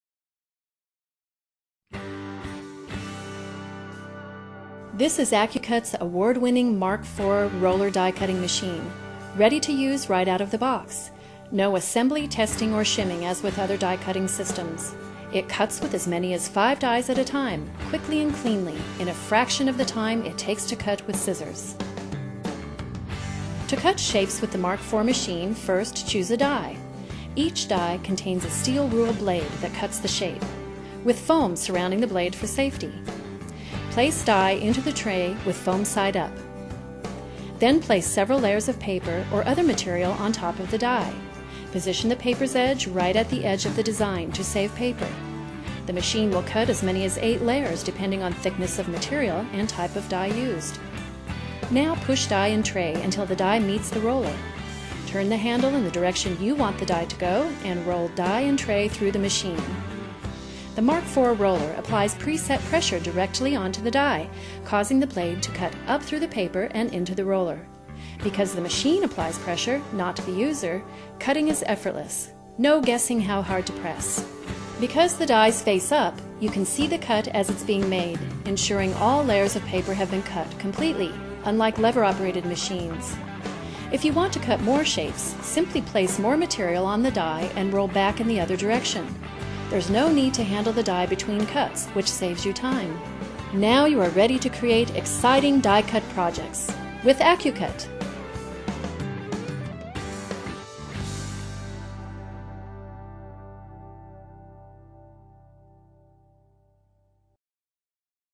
AccuCut Mark IV.flv
How to use die cutting machine with patented roller action and 12" cutting area.